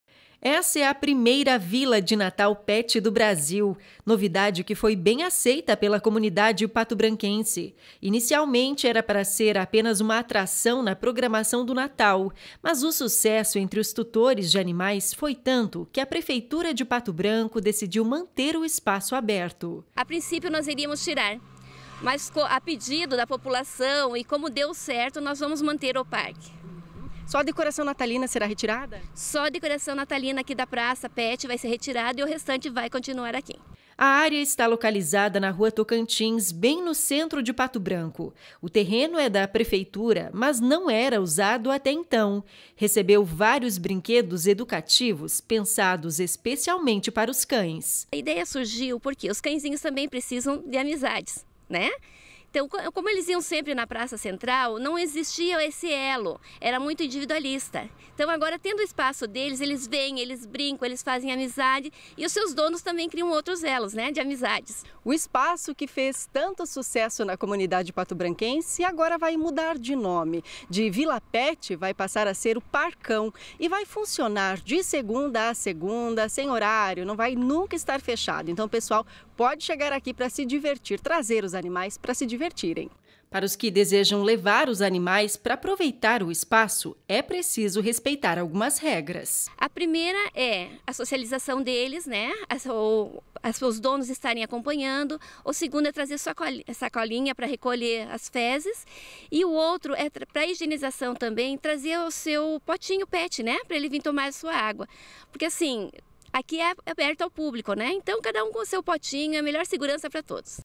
Vila Pet seguirá aberta
Atualidade: A Vila Pet inaugurada durante a programação de Natal em Pato Branco vai continuar aberta. O espaço agora será chamado de “Parcão” e segue a disposição de tutores e animais.